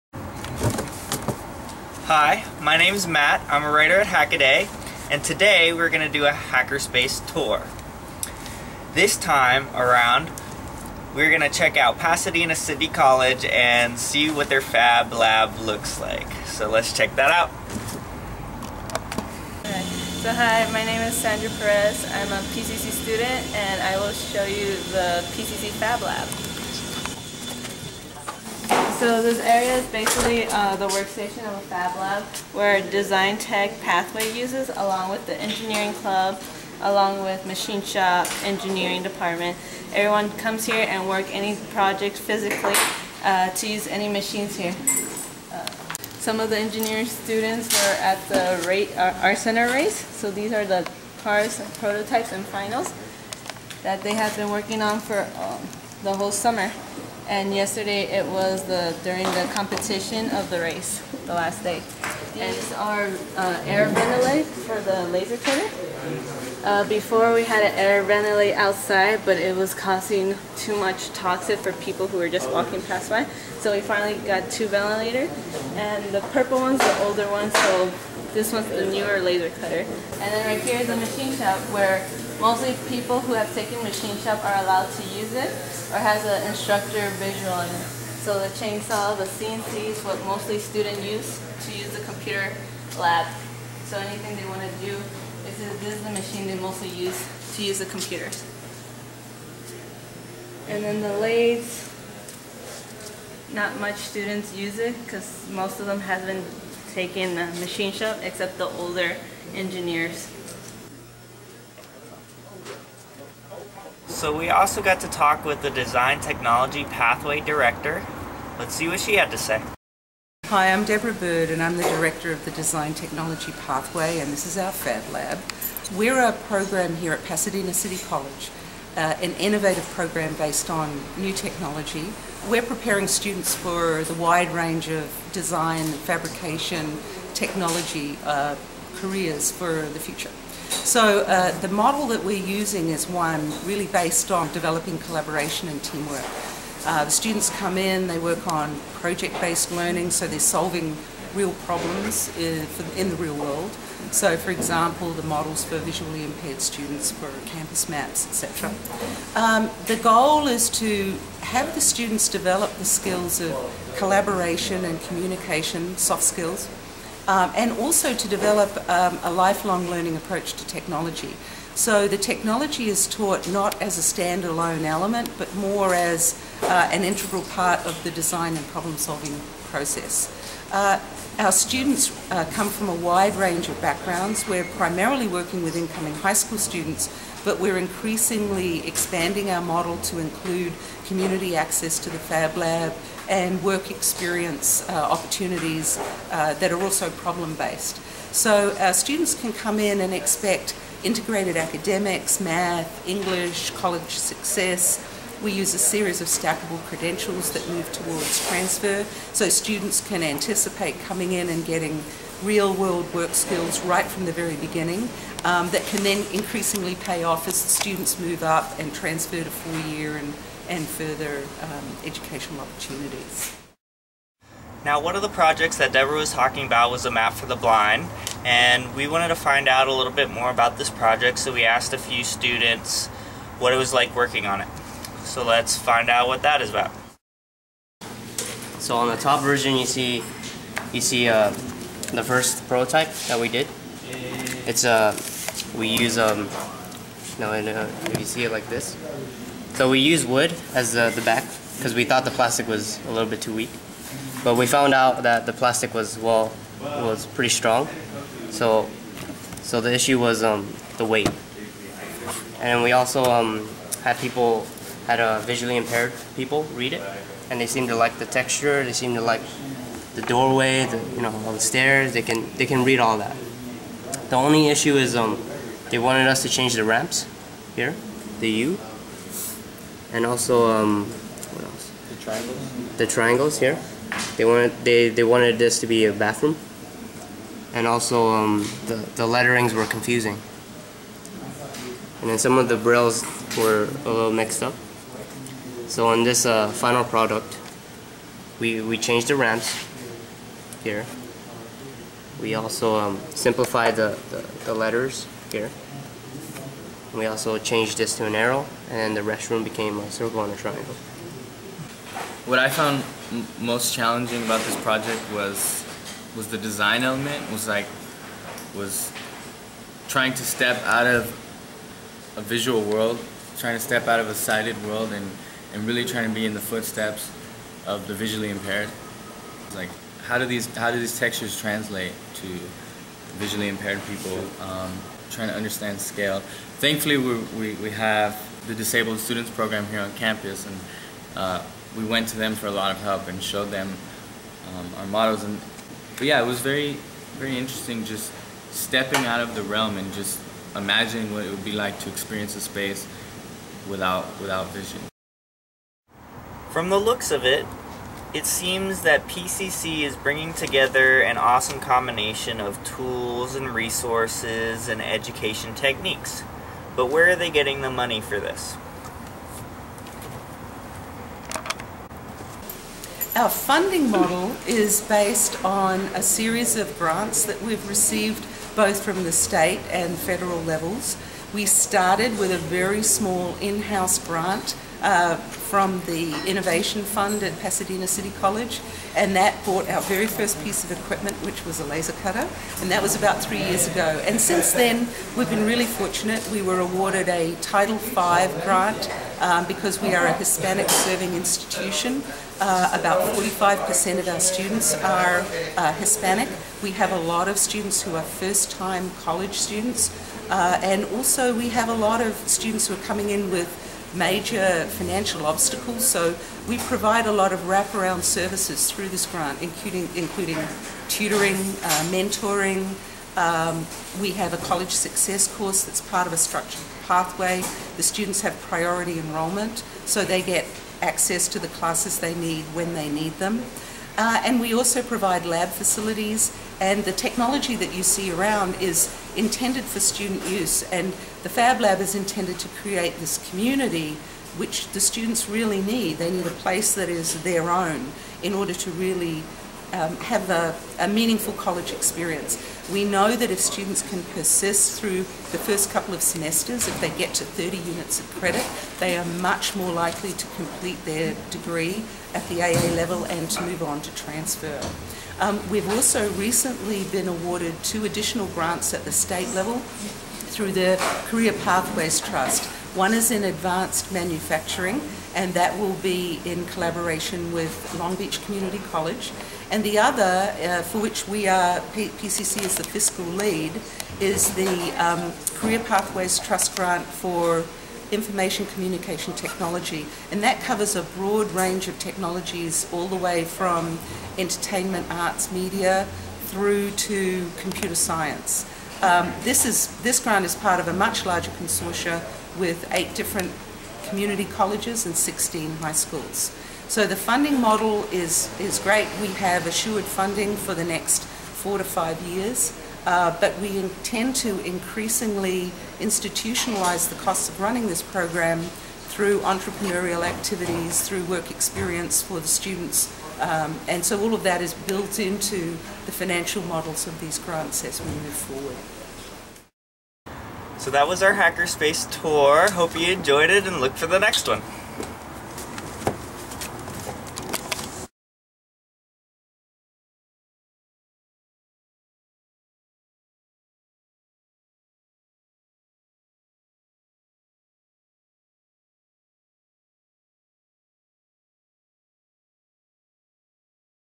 Fab Lab Tours: Pasadena City College Fab Lab
This time around we check out the cool stuff happening at PCC, in California. We show interviews with the Director of the Design Technology Pathway along with students working the space. Featured in this video is a 3D Printed map for the blind.

Filmed and edited by Matthew Terndrup